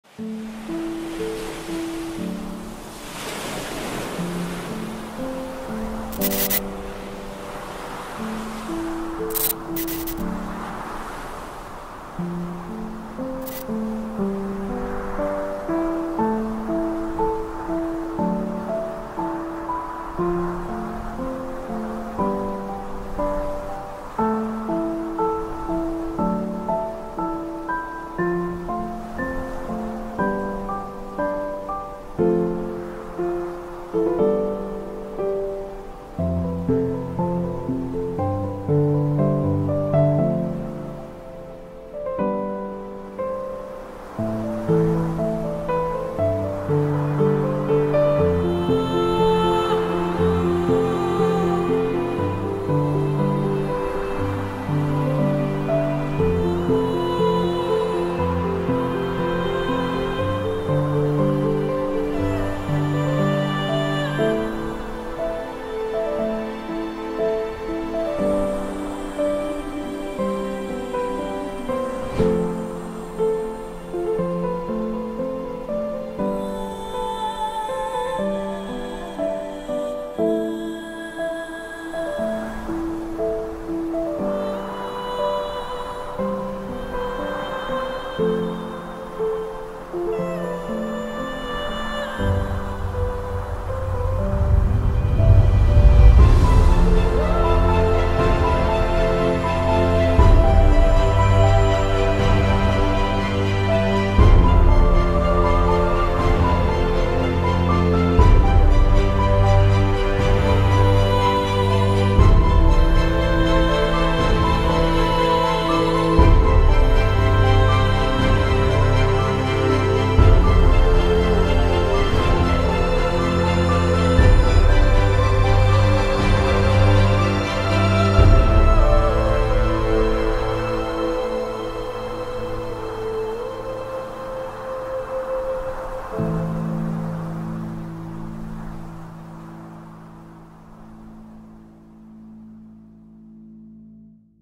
Bird's eye view Kitefoiling on Bonefish Bay, San Salvador, The Bahamas
Aerial drone shots of Bonefish Bay, San Salvador, The Bahamas.